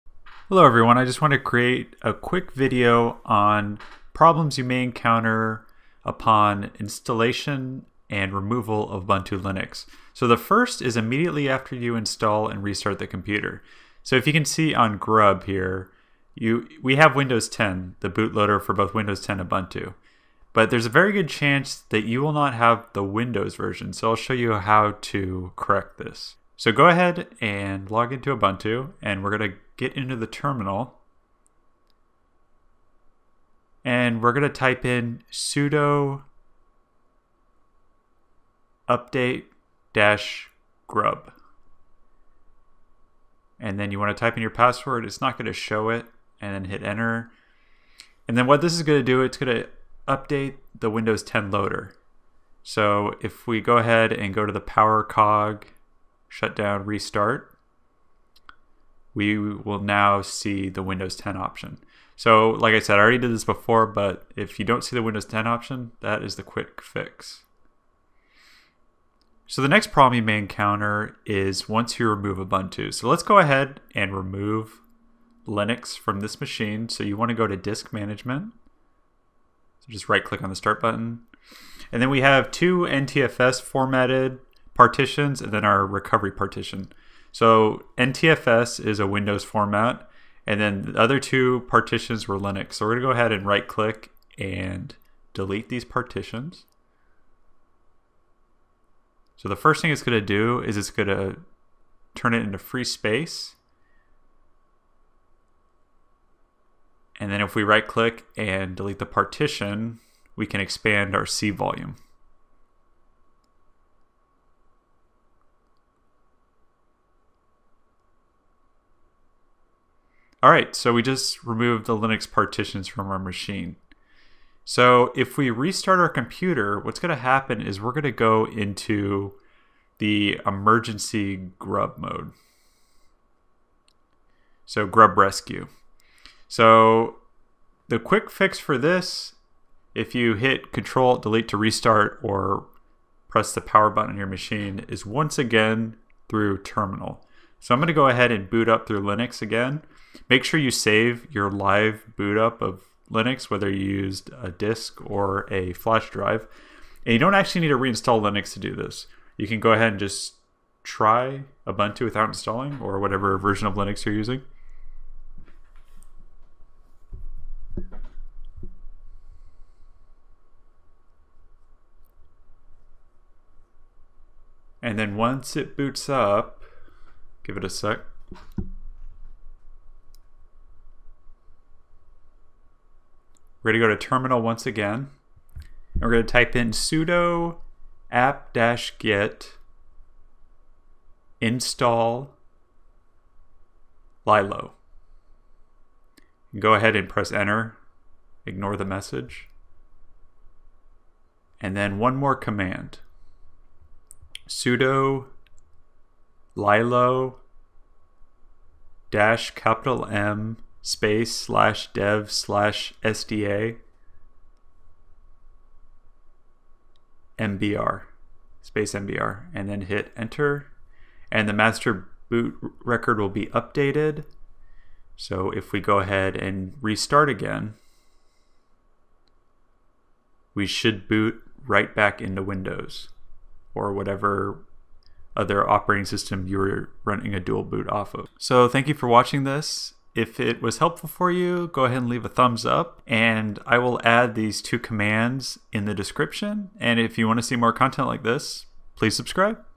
GRUB Bootloader Fix: How to Boot in to Windows After Installing/Deleting Ubuntu Linux
Get The Complete Linux Administration Course Bundle!

After installing Ubuntu Linux, type the following commands in to the terminal to updated GRUB and to see the option to boot in to Windows:

sudo update-grub

After deleting the Linux partitions from your hard drive, do a live boot in to Linux (Try Ubuntu without installing) and type the following command in to the terminal to boot back in to Windows:

sudo apt-get install lilo

Hit enter then:

sudo lilo -M /dev/sda mbr